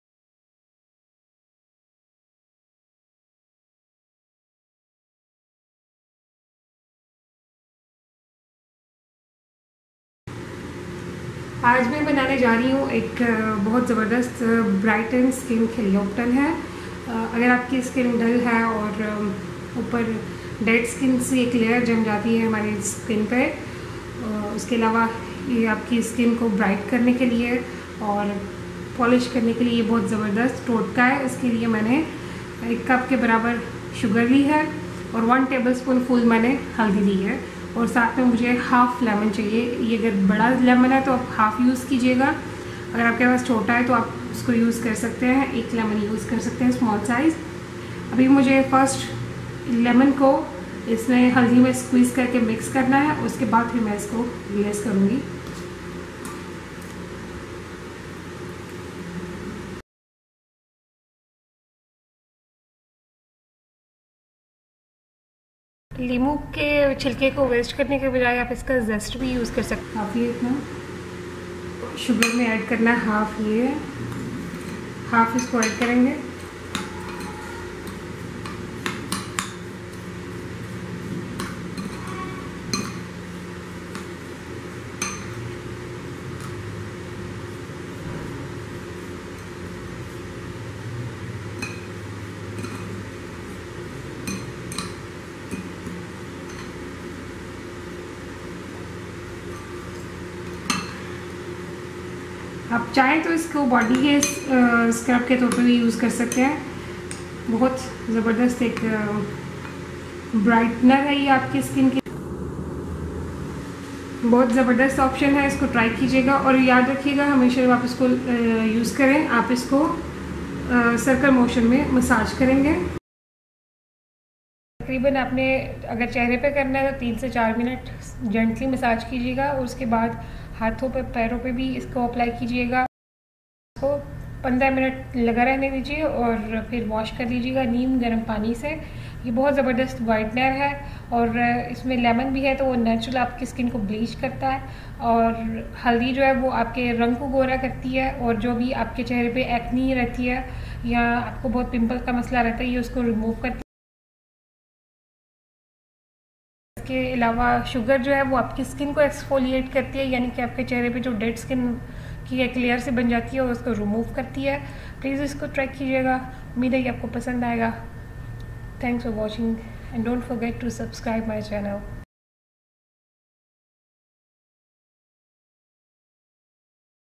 HOME MADE SCRUB TO BRIGHTAN,CLEAR DULL SKIN
USE 1 TABLE SPOON REST OF PRODUCT KEEP IN FRIDGE FOR MORE NEED